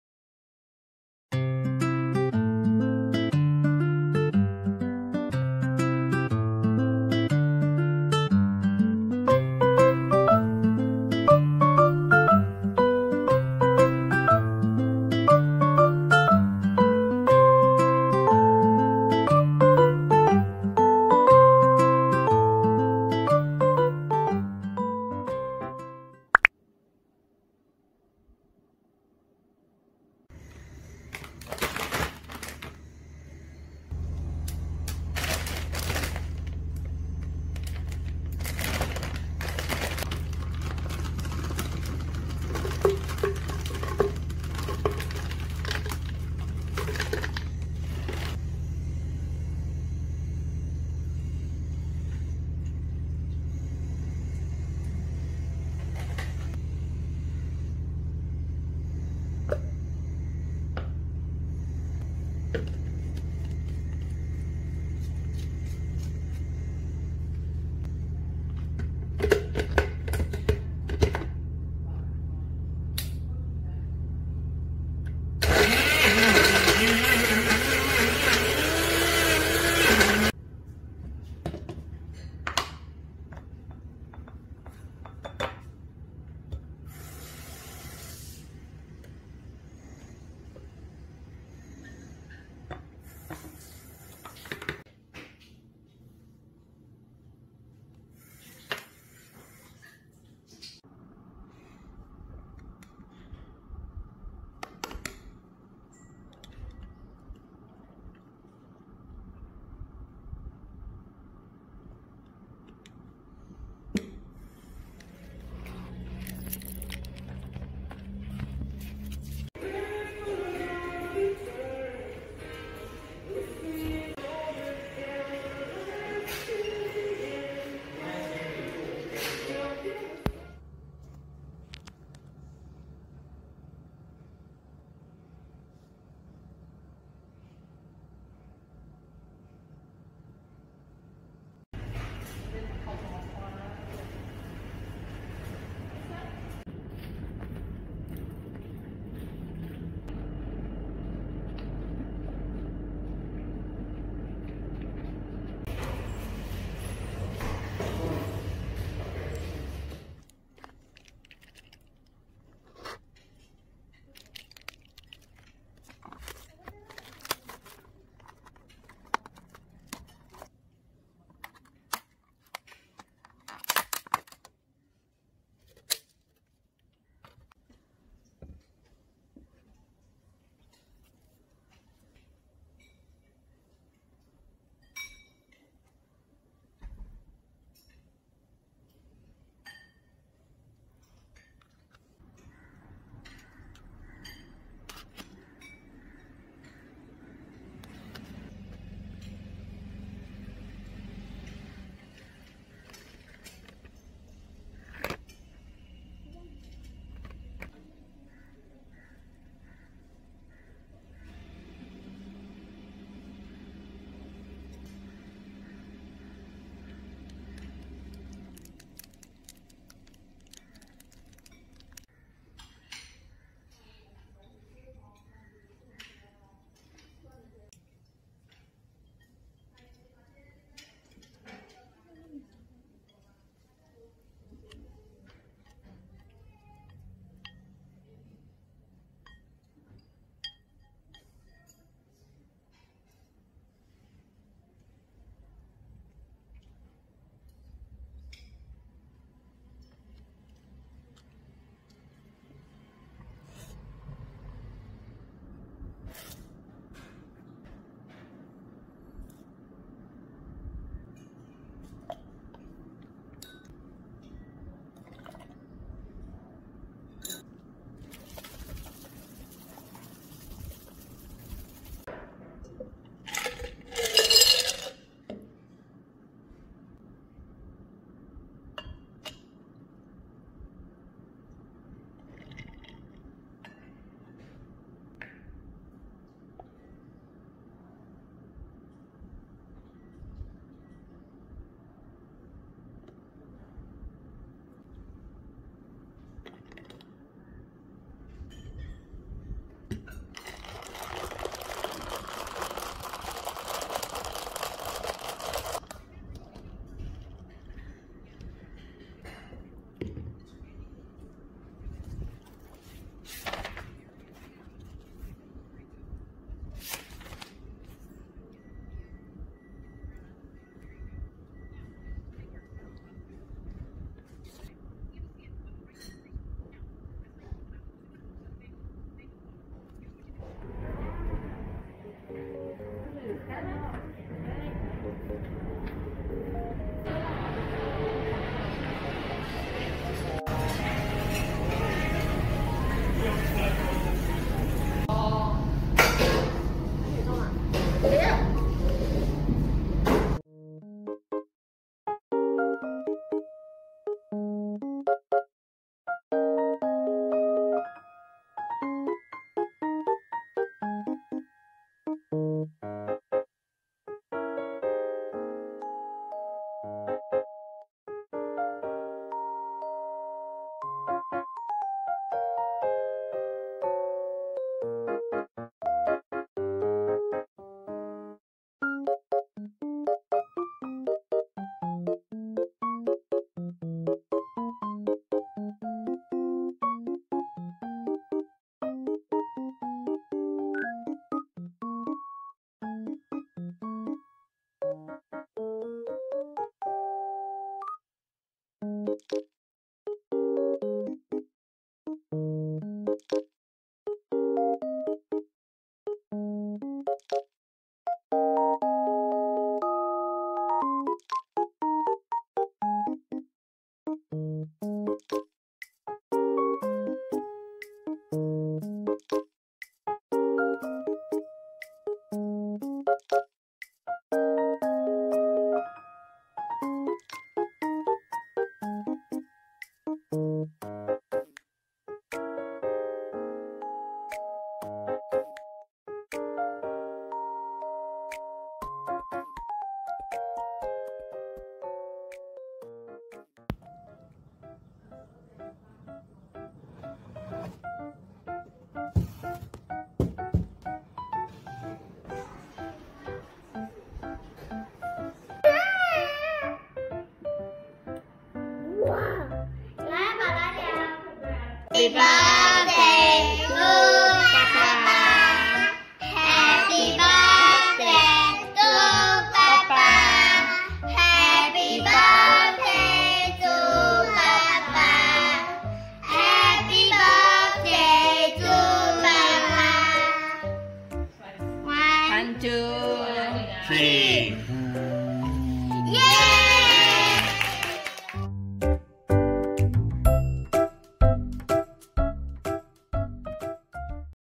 Vlog| Sashimi don, dalgona thai ice tea and celebrate my brother's birthday 🎂!
Highlight of my week is that I finally cut my hair! I also made dalgona thai iced tea and ate sashimi don 😍. I celebrated my brother's birthday with my family too. Do watch the vlog to find out more 🥰

Places I went in this video:
*Secret Recipe
*Shinmapo (SS15 Courtyard)

-Sashimi purchased from Top Catch Fisheries (Bukit Jalil)